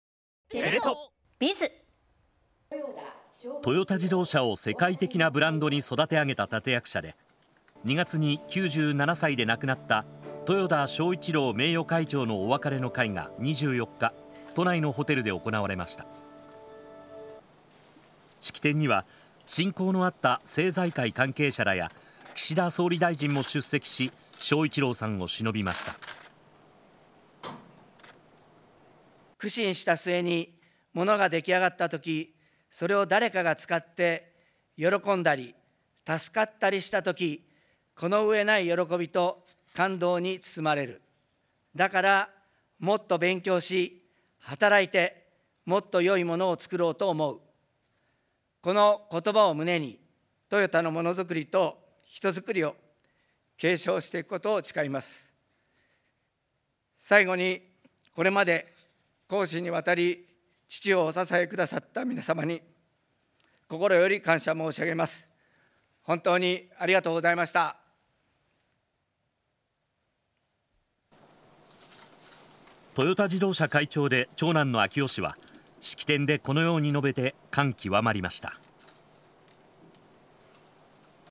豊田章一郎さんお別れの会（2023年4月24日）
トヨタ自動車を世界的なブランドに育て上げた立役者で、２月に97歳で亡くなった豊田章一郎名誉会長のお別れの会が24日、都内のホテルで行われました。式典には親交のあった政財界関係者らや、岸田総理大臣も出席し、章一郎さんを偲びました。
豊田章男会長「苦心した末にモノが出来上がったとき、それを誰かが使って喜んだり、助かったりしたとき、この上ない喜びと感動に包まれる。だからもっと勉強し、働いて、もっと良いモノをつくろうと思う。この言葉を胸にトヨタのモノづくりと人づくりを継承していくことを誓います」
トヨタ会長で長男の章男氏は、式典でこのように述べて感極まりました。